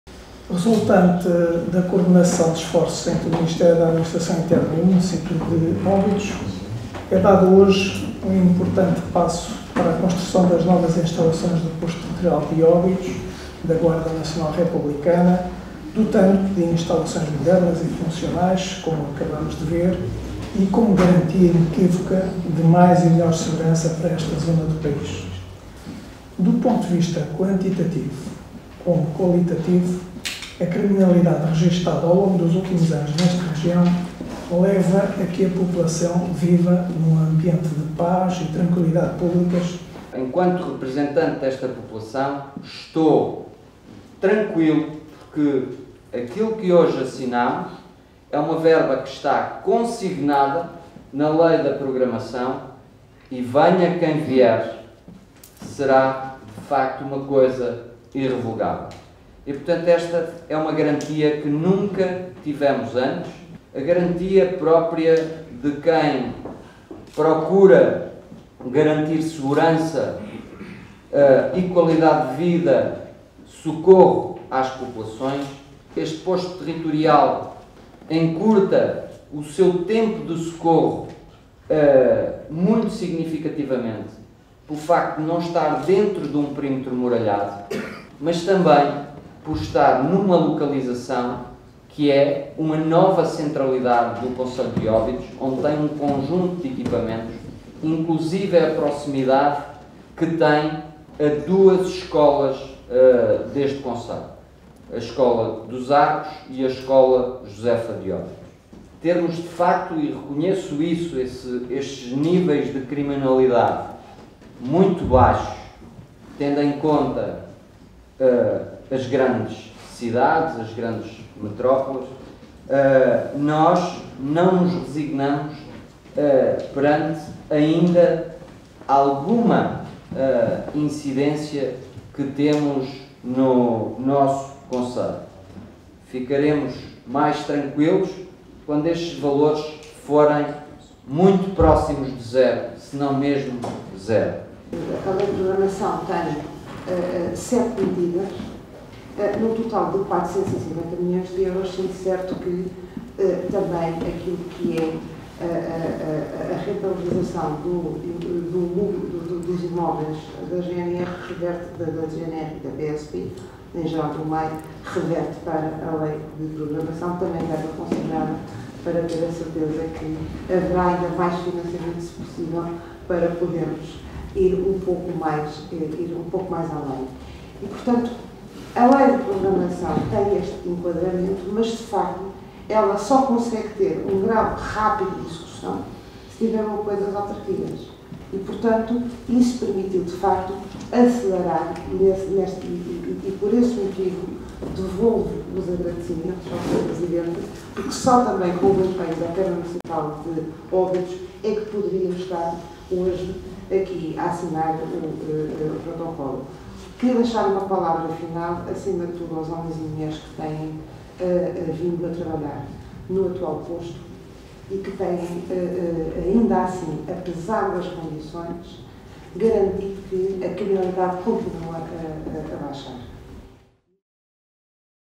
Assinatura do contrato para a construção do novo Quartel da GNR
Foi assinado, no passado dia 29 de Julho, em Óbidos, o contrato que vai permitir a construção do novo quartel da GNR de Óbidos. Novas instalações, localizadas junto ao Complexo Desportivo Municipal, que vão permitir encurtar o tempo de socorro às populações